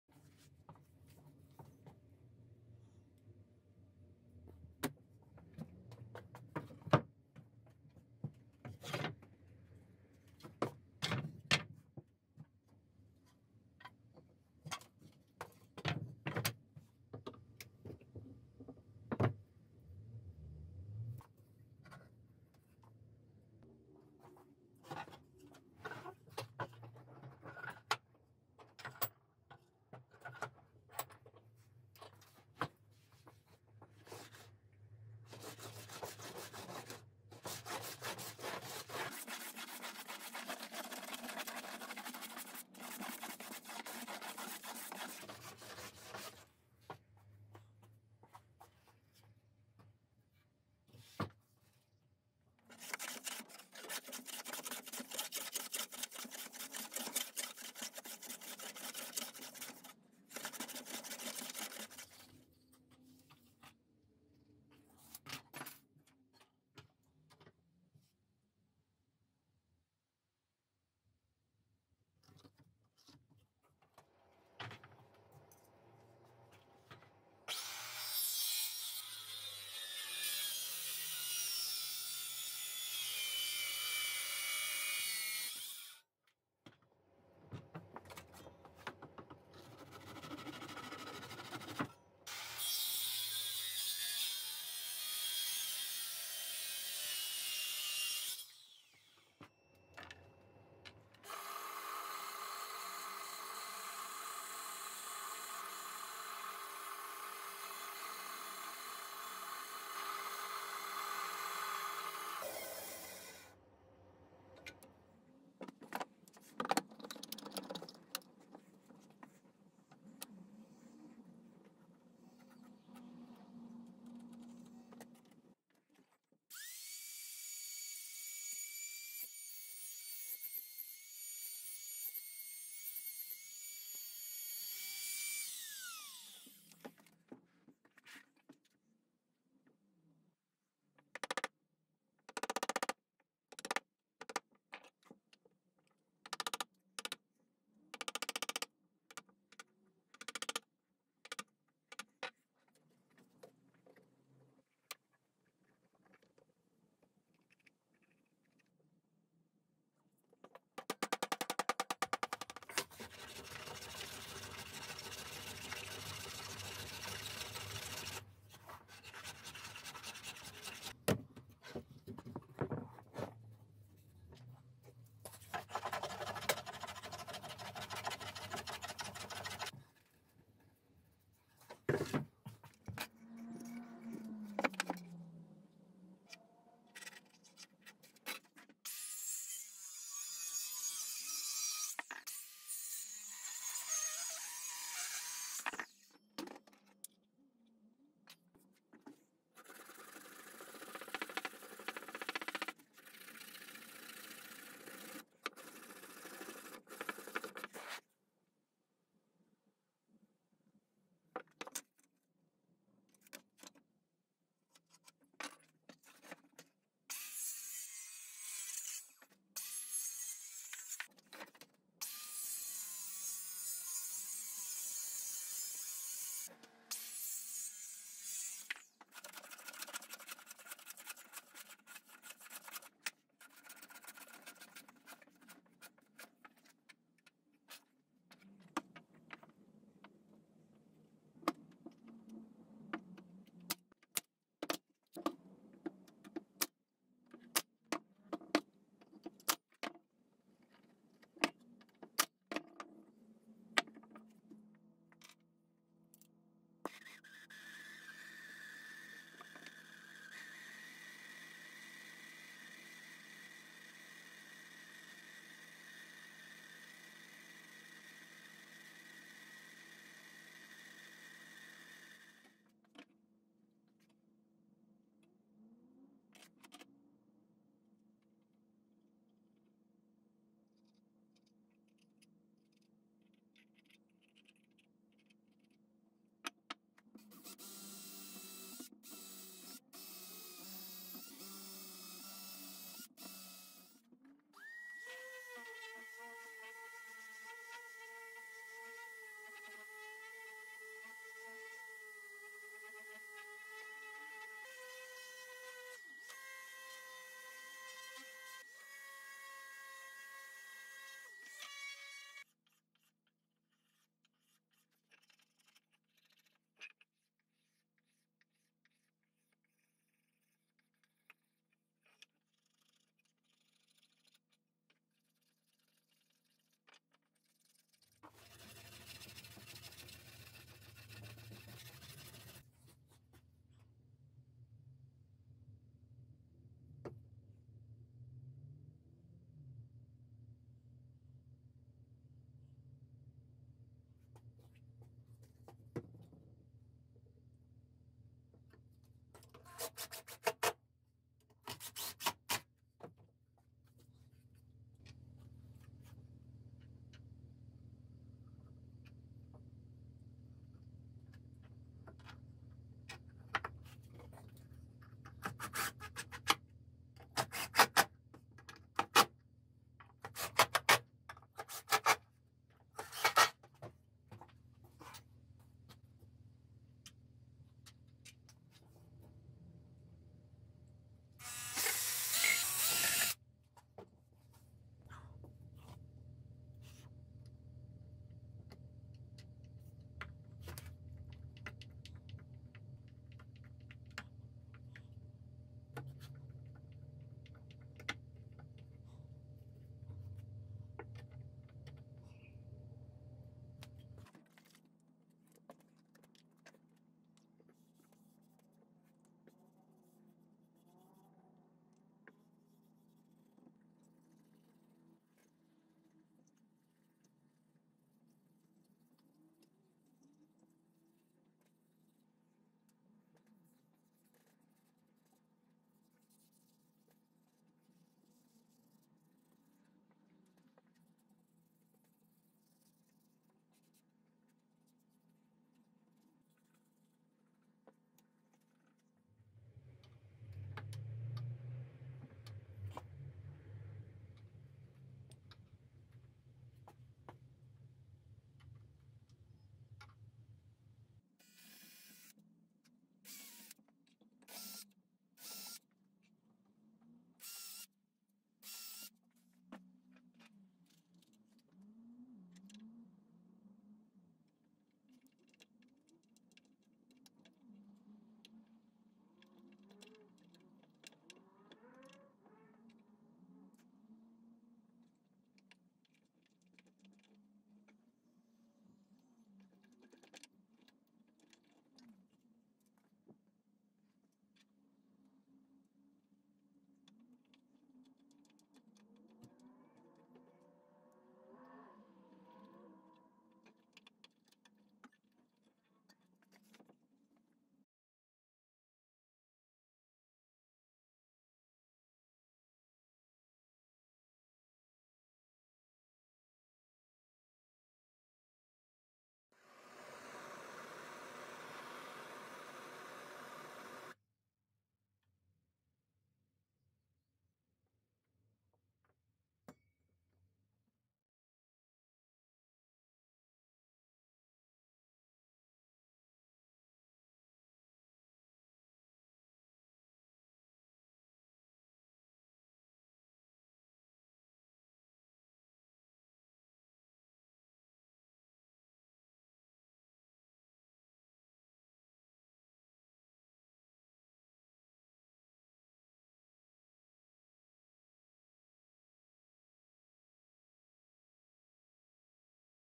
Entryway Organiser from pallet wood - Good Idea for your Home
In this video im making entryway organiser requested by my lovely girl.

For this project im using pallet wood, old keys and simple coat hooks.